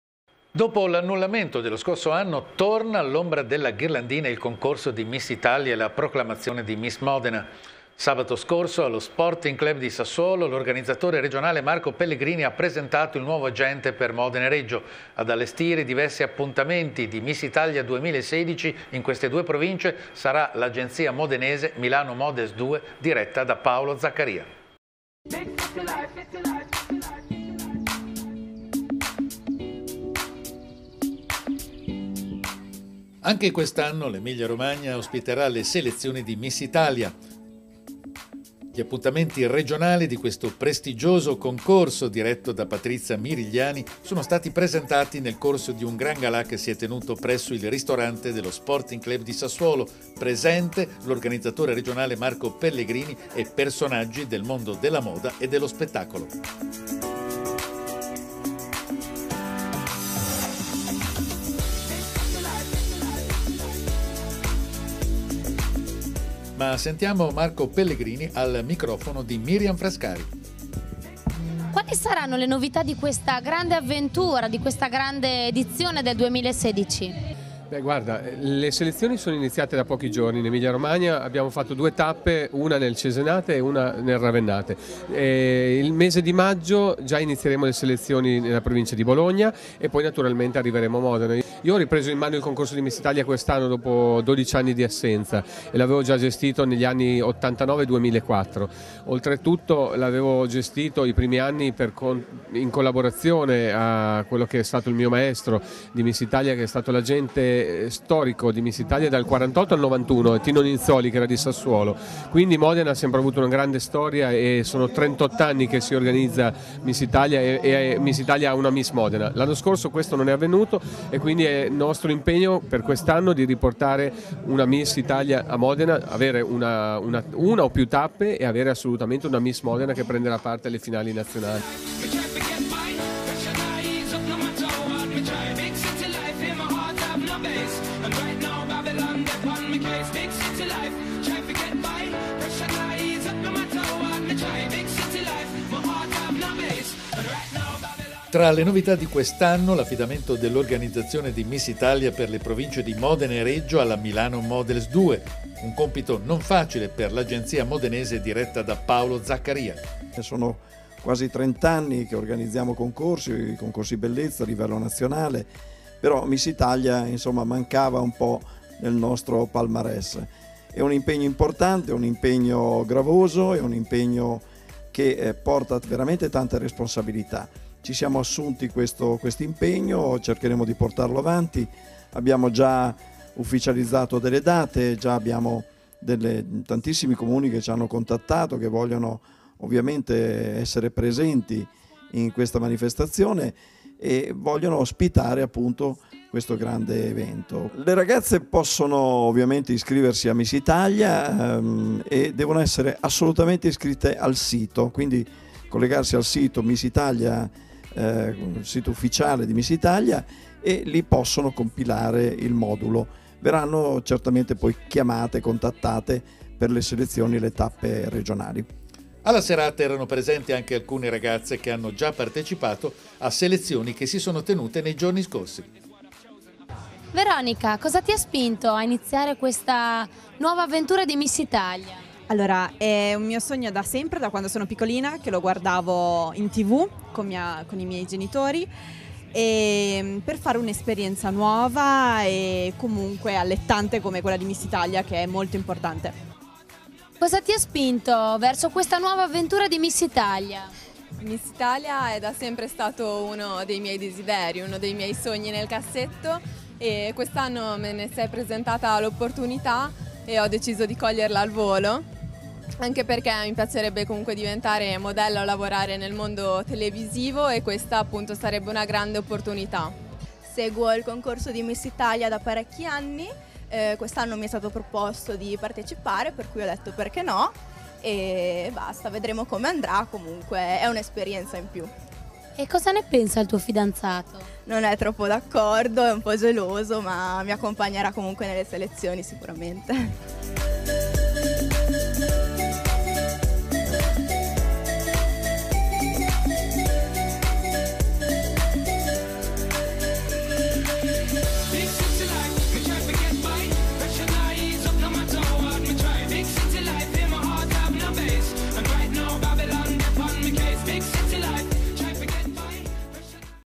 Presentazione "Miss Italia 2016" per le provincie di Modena e Reggio - 19.03.2016
Il servizio, realizzato da Claudio Maria Goldoni con le interviste di Miriam Frascari, in onda nel Tg di TeleStudio Modena mercoledì 23 marzo 2016, è stato effettuato in occasione della presentazione del nuovo agente di "Miss Italia" per le provincie di Modena e Reggio Emilia che per l'edizione 2016 sarà l'agenzia modenese "Milano Models 2" diretta da Paolo Zaccaria. All'evento era presente  Marco Pellegrini, organizzatore di Miss Italia 2016 per la regione Emilia-Romagna.  La serata si è tenuta sabato 19 marzo 2016 nel salone dello Sporting Club di Sassuolo (provincia di Modena). Nel servizio, le interviste realizzate a Marco Pellegrini, Paolo Zaccaria e alle aspiranti "Miss Italia" Veronica  Rudnicki, Veronica Fantuzzi e Alessia Consolini, concorrenti che hanno già partecipato ad alcune selezioni che si sono tenute in Romagna.